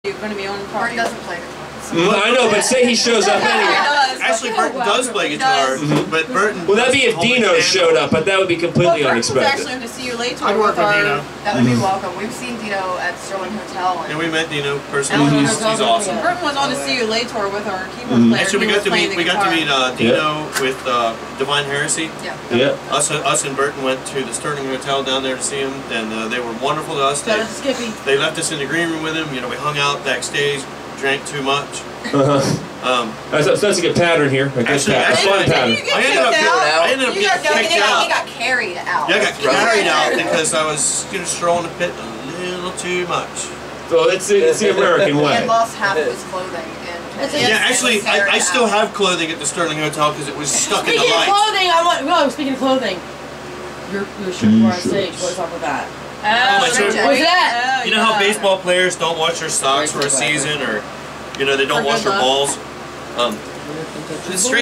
KMX Interviews Pulled Under on 191 Live Part 1
KMX Finds Out More Than He Wants To Know :)